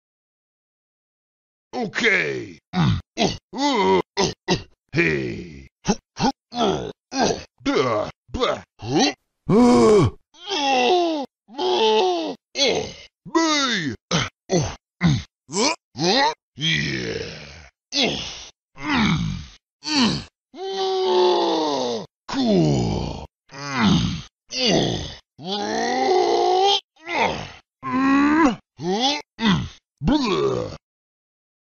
S O U N D S - O F - D K
I found a collection of DK sound clips from the different voice actors for him, and I decided to make this from Grant Kirkhope's DK voice clips.

(Original Video ... though, as of 3/20/2023, the original has been deleted and replaced with this newer version ... but that ones deleted too as of 2/23/2024, so now here's an even newer version